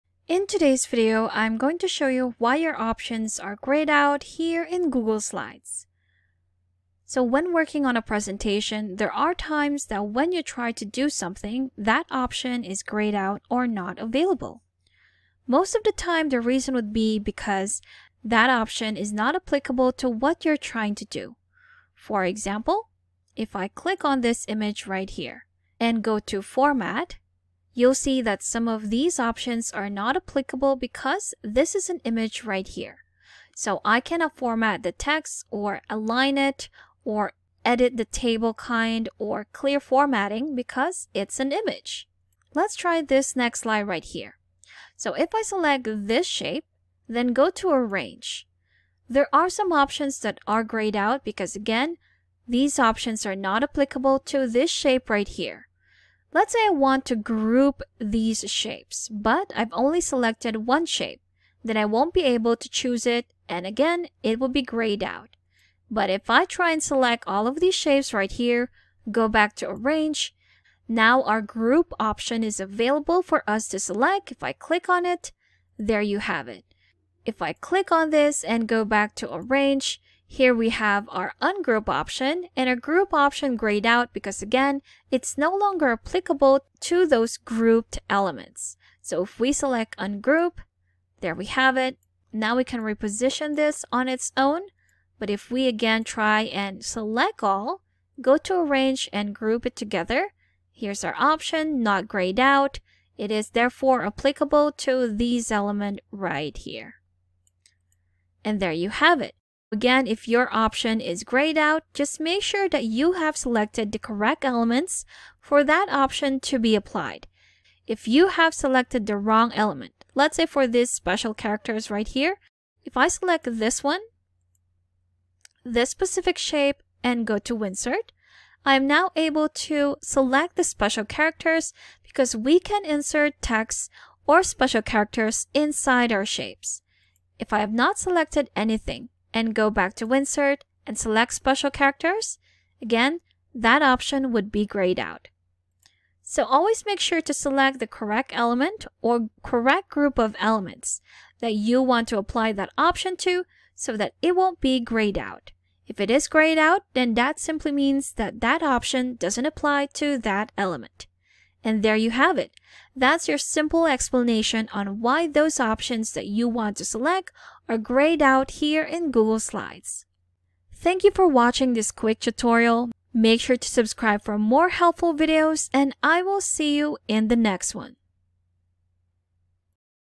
Google Slides | Why is the option Greyed out? (Quick Explanation)
Whether you're trying to edit text, insert elements, or access specific features, I'll walk you through common causes and solutions. Learn how permissions, file formats, and editing modes impact your ability to make changes. I’ll also cover troubleshooting steps to regain full control of your slides. If you've ever been frustrated by disabled options in Google Slides, this guide will help you understand and resolve the issue quickly.